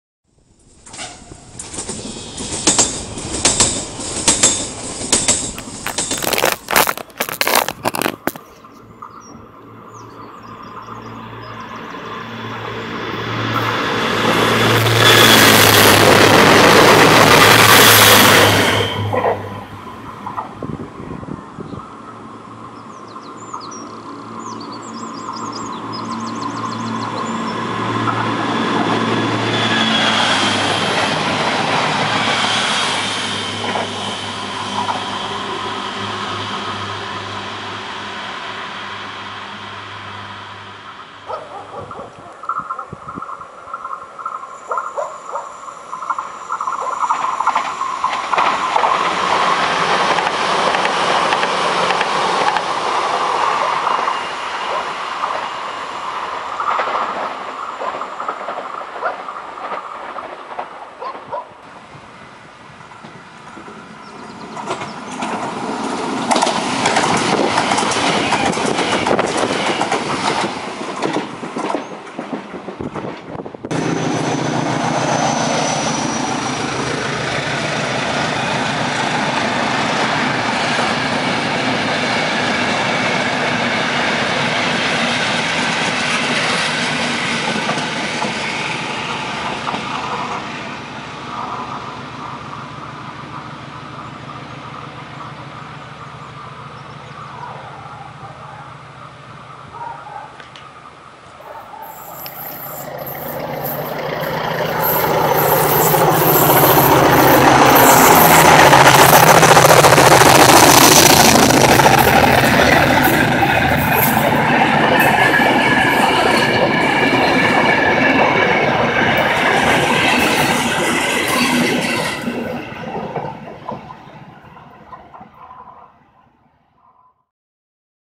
0794 🇵🇹 Cabeda 🚆 CP Movements ● Compilation
Line: Douro (Porto ↔ (Ermesinde ↔ Pocinho))
Place: Cabeda, Alfena, Valongo, Porto/Douro Litoral, Portugal
Date and Time: 2016 apr (12:34 p.m.)
Service: Commuter & Inter-regional
Rolling Stock: CP 592.0 unit 059,  CP 3400 (units 34** e 3424) & CP 1424 + Dyf408 nº F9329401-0 + A7yf 704 nº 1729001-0 + SRyf2 nº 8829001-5 + Sy3 nº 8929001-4 + Sy4 nº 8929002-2 + Sy5 nº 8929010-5

Linha do Douro (Porto ↔ (Ermesinde ↔ Pocinho))
Localização: Cabeda, Alfena, Valongo, Porto/Douro Litoral, Portugal
Data: 2016 abril
Hora: 17:34
Serviço:  Inter-regional 878 (Régua ➙ Porto São Bento) & outros serviços regionais
Material Circulante: CP 592.0 unidade 059,  CP 3400 (unidades 34** e 3424) & CP 1424 + Dyf408 nº F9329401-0 + A7yf 704 nº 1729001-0 + SRyf2 nº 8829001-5 + Sy3 nº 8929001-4 + Sy4 nº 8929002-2 + Sy5 nº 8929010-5  (comboio presidencial)

Linha do Douro